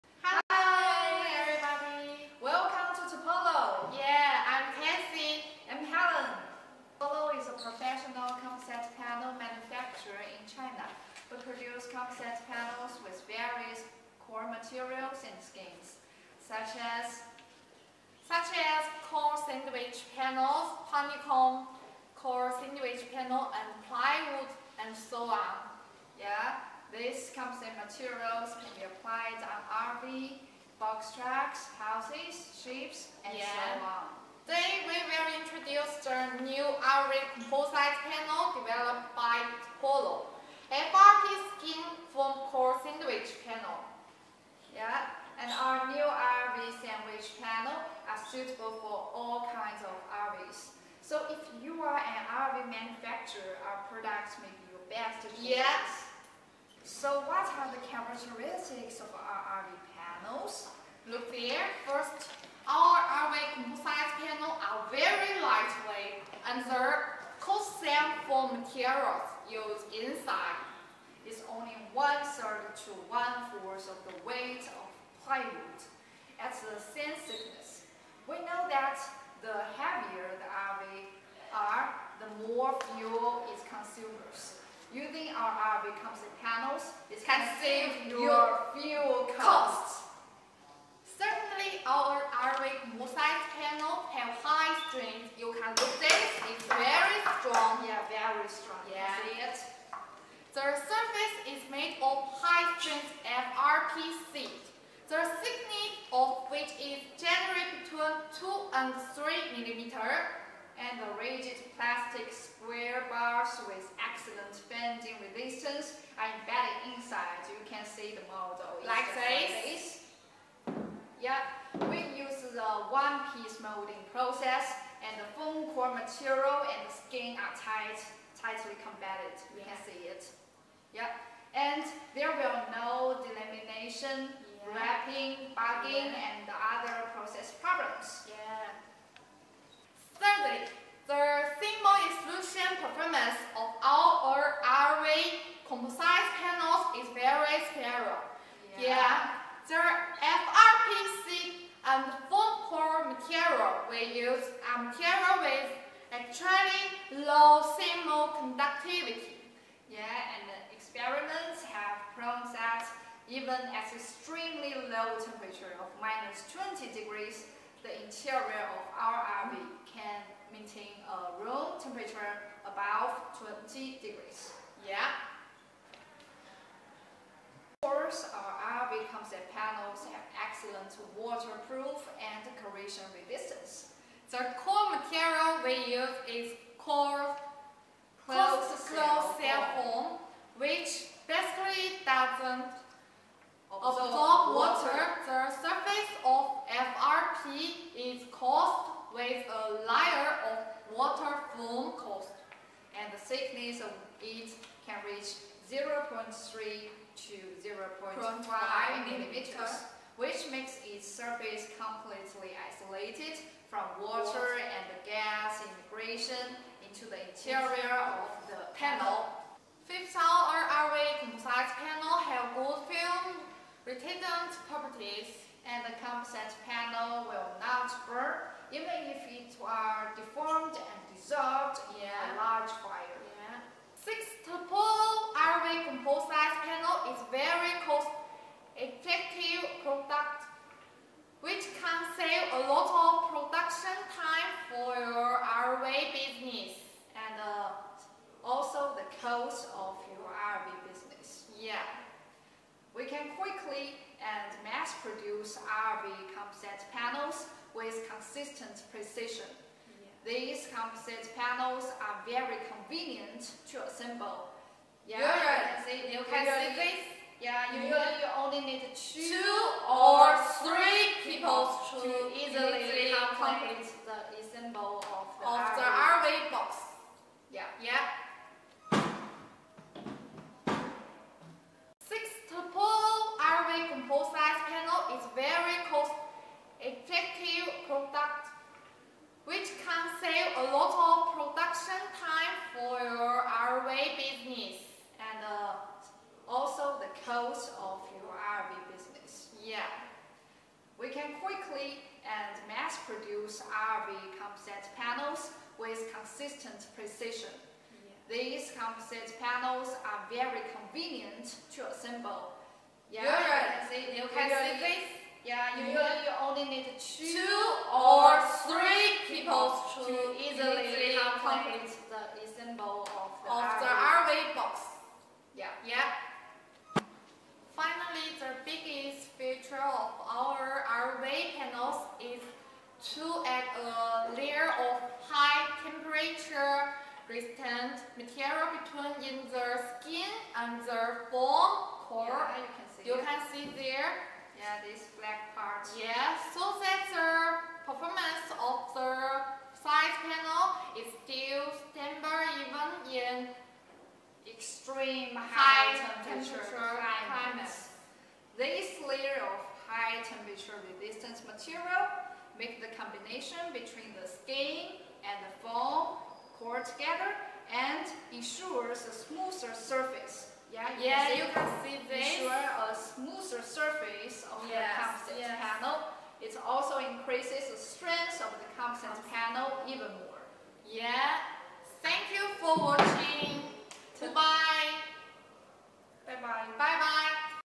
Advantages of TOPOLO New RV Composite Panels
What are the characteristics of our RV composite panels?
First, our caravan composite panels are very lightweight, and the closed-cell foam material used inside it is only one-third to one-fourth of the weight of plywood at the same thickness. We know that the heavier the RV body, the more fuel it consumes. Using our RV composite panels can save you fuel costs.
Secondly, our caravan composite panels have high strength. The surface is made of high-strength FRP sheets, the thickness of which is generally between 2 and 3 mm, and rigid plastic square bars with excellent bending resistance are embedded inside. We use the one-piece molding process, the foam core material and the skin are tightly combined, and there will be no delamination, warping, bulging and other process problems.
Third, the thermal insulation performance of our RV composite panels is very superior. The FRP sheet and foam core material we use are materials with extremely low thermal conductivity. Experiments have proved that even at the extremely low temperature of minus 20 degrees Celsius, the interior of the RV can maintain a room temperature above 20 degrees Celsius.
Fourth, our RV composite panels have excellent waterproof and corrosion resistance. The core material we use is closed-cell foam, which basically does not absorb water; the surface of FRP is coated with a layer of waterproof gel coat, the thickness of which can reach 0.3~0.5mm, which makes its surface completely isolated from water and gas immersion into the interior of the composite panel.
Fifth, our RV composite panels have good flame-retardant properties, and the composite panels will not burn even if they are deformed and dissolved in a large fire.
Sixth, TOPOLO's RV composite panel is a very cost-effective product, which can save a lot of production time for your RV business. We can quickly and mass-produce RV composite panels with consistent precision. These composite boards are very convenient to assemble. Usually, you only need 2 or 3 people to easily complete the assembly of a RV box.
Finally, the biggest feature of our caravan composite panel is to add a layer of high-temperature resistant material between the skin and the foam core, so that the performance of the composite panel is still stable even in extreme high-temperature climates. This layer of high-temperature-resistant material makes the combination between the skin and the foam core tighter and ensures a smoother surface of the composite panel. It also increases the strength of the composite panel even more.